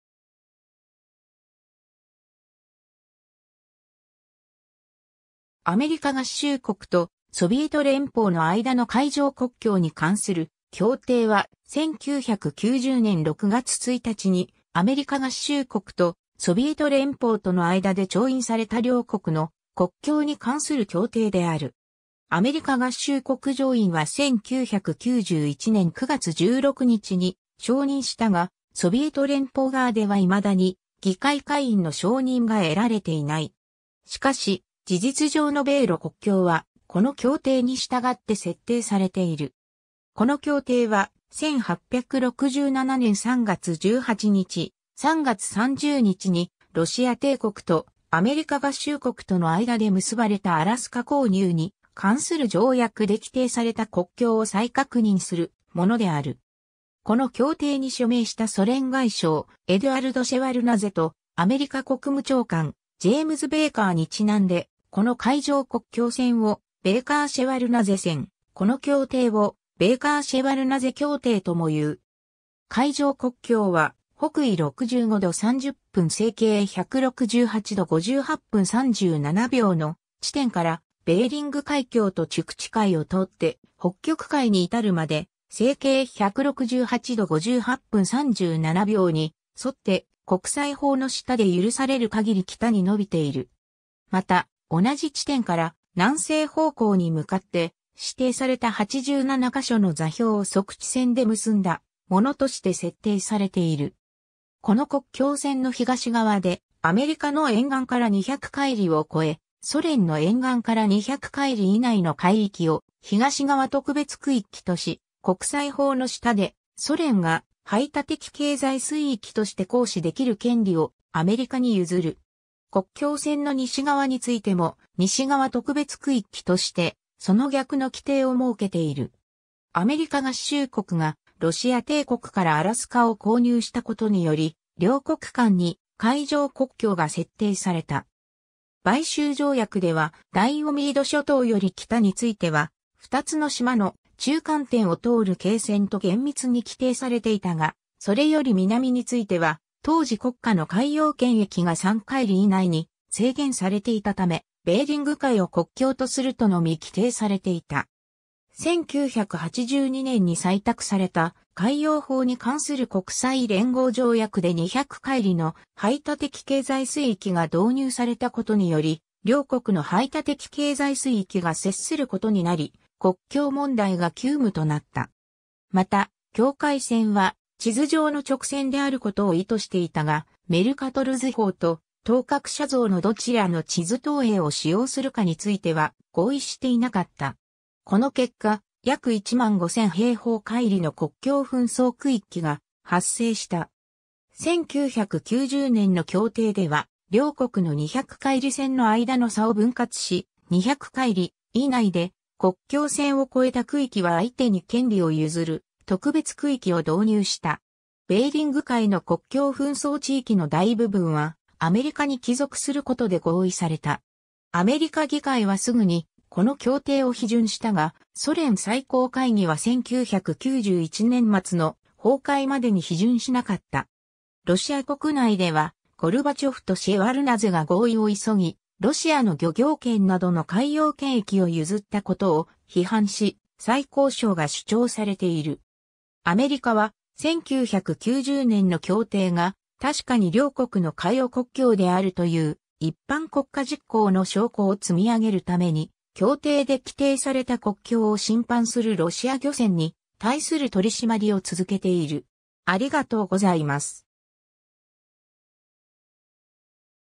アメリカ合衆国とソビエト連邦の間の海上国境に関する協定
アメリカ合衆国とソビエト連邦の間の海上国境に関する協定（アメリカがっしゅうこくとソビエトれんぽうのあいだのかいじょうこっきょうにかんするきょうてい、Agreement between the United States of America and the Union of Soviet Socialist Republics on the maritime boundary）は、1990年6月1日にアメリカ合衆国とソビエト連邦との間で調印された両国の国境に関する協定である。
 アメリカ合衆国上院は1991年9月16日に承認したが、ソビエト連邦（およびその後継国家であることを宣言したロシア連邦）側では未だに議会下院の承認が得られていない。
しかし、事実上の米露国境はこの協定に従って設定されている。
 この協定は、1867年3月18日（ユリウス暦）/3月30日（グレゴリオ暦）にロシア帝国とアメリカ合衆国との間で結ばれたアラスカ購入に関する条約で規定された国境を再確認するものである。
この協定に署名したソ連外相エドゥアルド・シェワルナゼとアメリカ国務長官ジェイムズ・ベイカーにちなんで、この海上国境線をベイカー・シェワルナゼ線、この協定をベイカー・シェワルナゼ協定ともいう。
 海上国境は、北緯65度30分西経168度58分37秒の地点から、ベーリング海峡とチュクチ海を通って北極海に至るまで、西経168度58分37秒に沿って国際法の下で許される限り北に延びている。
また、同じ地点から南西方向に向かって、指定された87箇所の座標を測地線で結んだものとして設定されている。
この国境線の東側（アメリカ側）で、アメリカの沿岸から200海里を超え、ソ連の沿岸から200海里以内の海域を「東側特別区域」とし、国際法の下でソ連が排他的経済水域として行使できる権利をアメリカに譲る。
国境線の西側についても「西側特別区域」としてその逆の規定を設けている。
 アメリカ合衆国がロシア帝国からアラスカを購入したことにより、両国間に海上国境が設定された。
買収条約では、ダイオミード諸島より北については「2つの島の中間点を通る経線」と厳密に規定されていたが、それより南については、当時国家の海洋権益が3海里以内に制限されていたため、ベーリング海を国境とするとのみ規定されていた。
1982年に採択された「海洋法に関する国際連合条約」で200海里の排他的経済水域が導入されたことにより、両国の排他的経済水域が接することになり、国境問題が急務となった。
また、境界線は地図上の直線であることを意図していたが、メルカトル図法と等角写像のどちらの地図投影を使用するかについては合意していなかった。
この結果、約15,000平方海里の国境紛争区域が発生した。
 1990年の協定では、両国の200海里線の間の差を分割し、200海里以内で国境線を越えた区域は相手に権利を譲る「特別区域」(special area)を導入した。
ベーリング海の国境紛争地域の大部分は、アメリカに帰属することで合意された。
アメリカ議会はすぐにこの協定を批准したが、ソ連最高会議は1991年末の崩壊までに批准しなかった。
ロシア国内では、ゴルバチョフとシェワルナゼが合意を急ぎ、ロシアの漁業権などの海洋権益を譲ったことを批判し、再交渉が主張されている。
アメリカは、1990年の協定が確かに両国の海洋国境であるという「一般国家実行」の証拠を積み上げるために、協定で規定された国境を侵犯するロシア漁船に対する取り締まりを続けている。